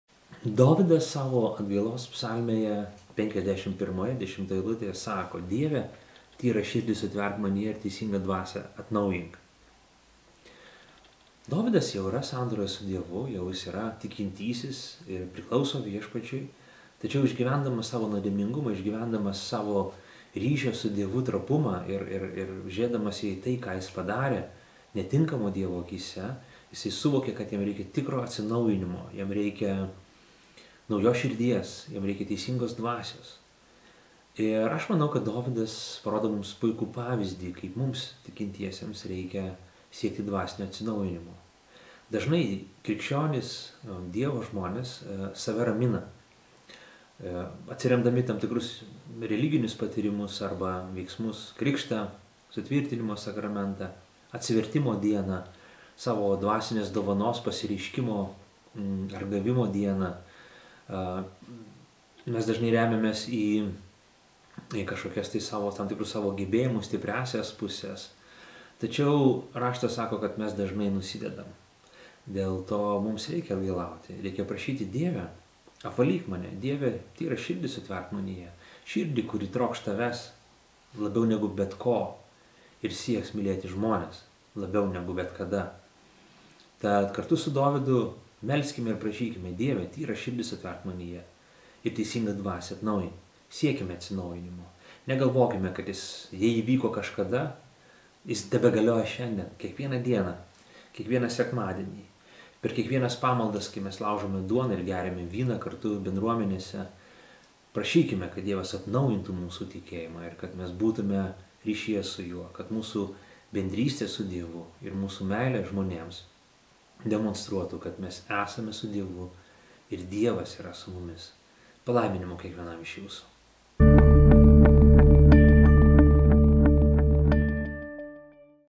Atgailaujanti dvasia
Karalius Dovydas siekė dvasinio atsinaujinimo, kai jis suvokė savo nuodėmingumą. Krikščionio gyvenimas yra nuolatinis dvasinis atsinaujinimas.
Psalmė 51:10